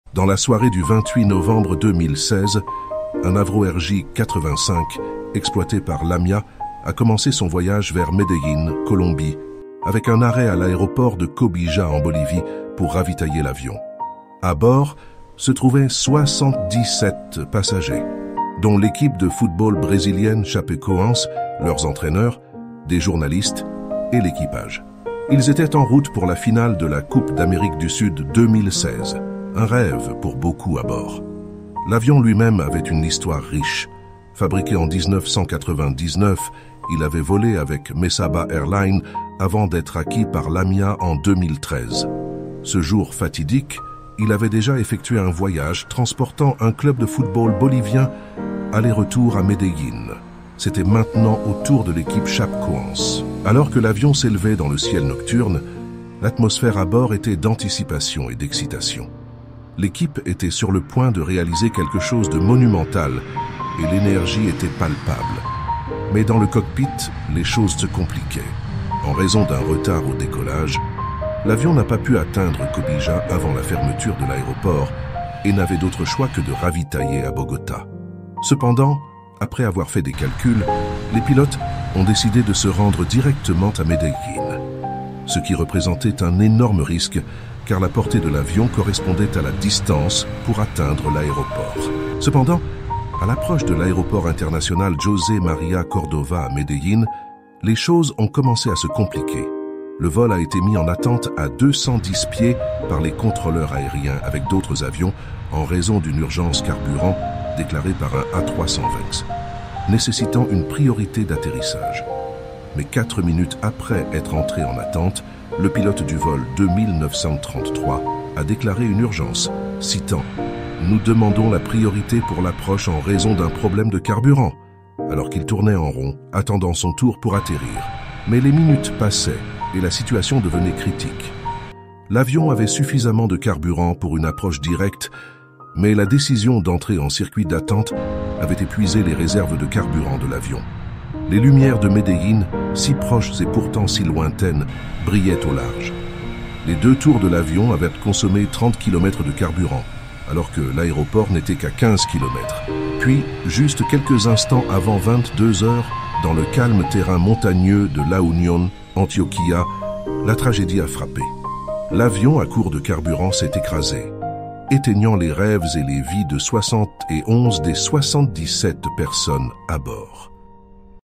Une Tragédie du football et Aérienne, le Mystère du Vol Lamia 2933.
Dans cette vidéo, nous plongeons dans l'histoire troublante du vol Lamia 2933, qui a tragiquement pris fin en 2016. Rejoignez-nous pour une analyse détaillée des événements qui ont conduit à cette catastrophe, des témoignages poignants et des leçons à tirer pour l'avenir de l'aviation.